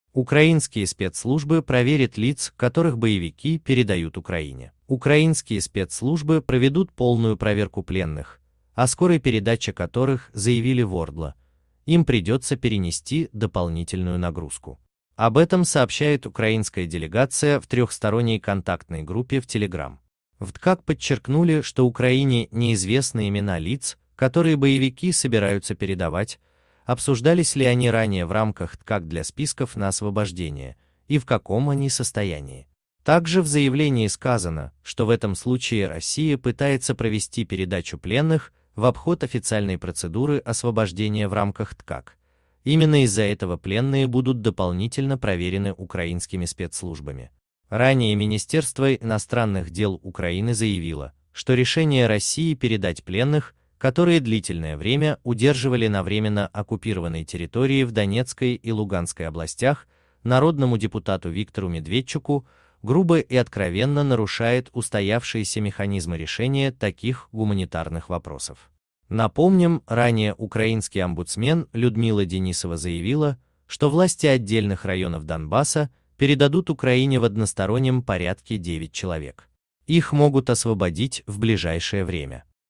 Украинские спецслужбы проверят лиц, которых боевики передают Украине.
Политика пленные Подробней на Украинские спецслужбы проведут полную проверку пленных, о скорой передаче которых заявили в ОРДЛО. Им придется перенести дополнительную нагрузку. Об этом сообщает украинская делегация в Трехсторонней контактной группе в Telegram. В ТКГ подчеркнули, что Украине неизвестны имена лиц, которые боевики собираются передавать, обсуждались ли они ранее в рамках ТКГ для списков на освобождение и в каком они состоянии. Источник